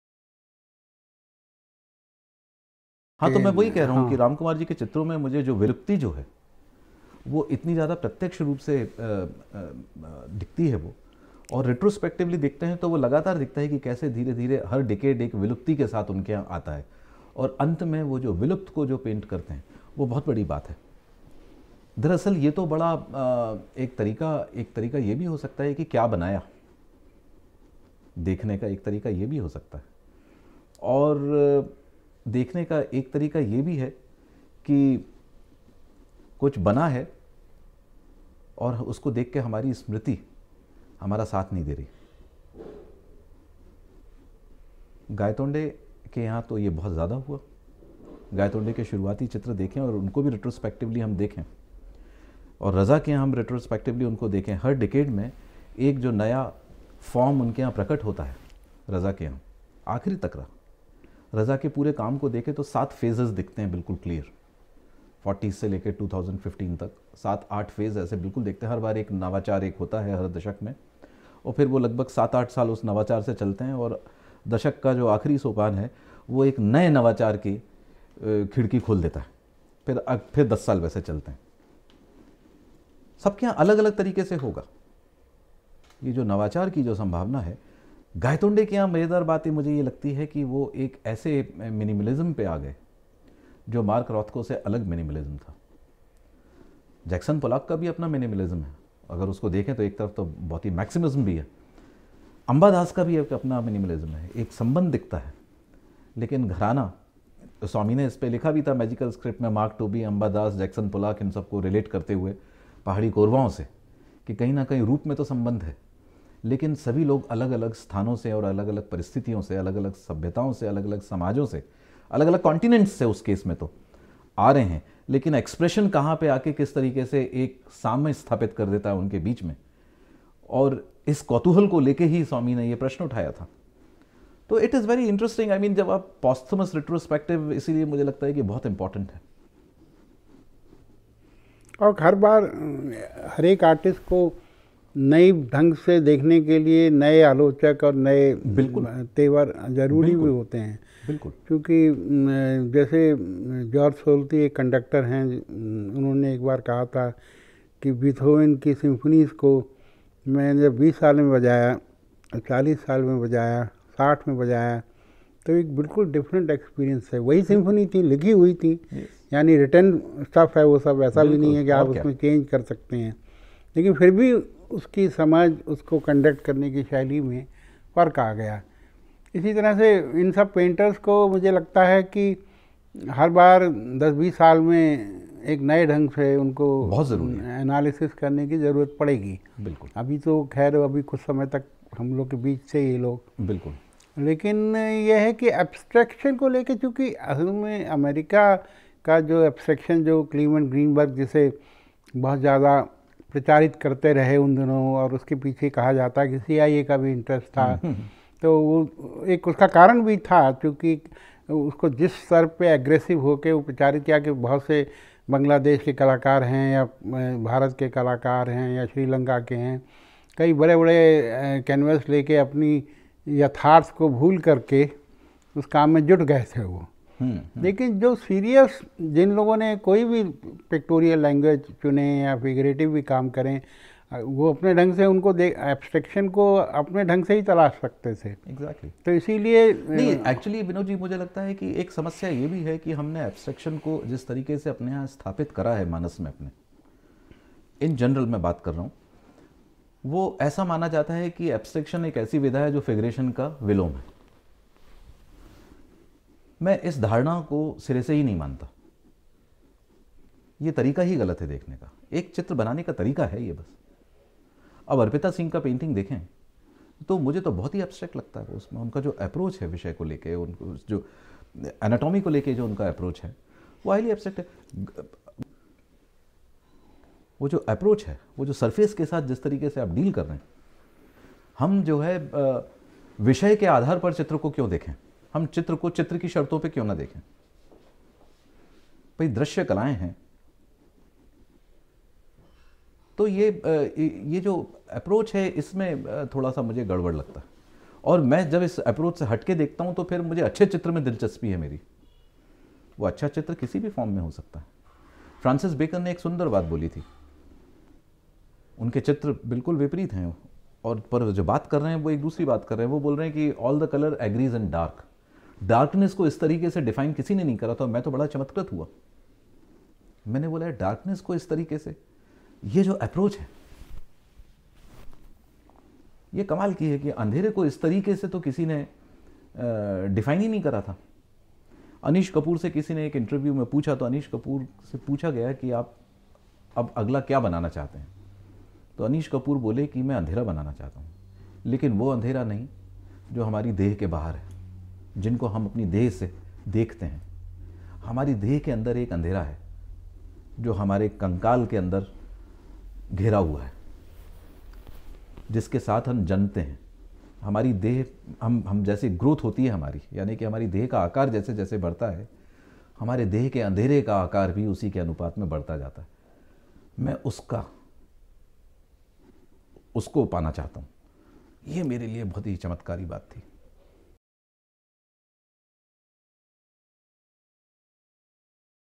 Manish Pushkale 3
Manish Pushkale
on How to Define Abstraction
Interview by Vinod Bhardwaj -3
 (Excerpts)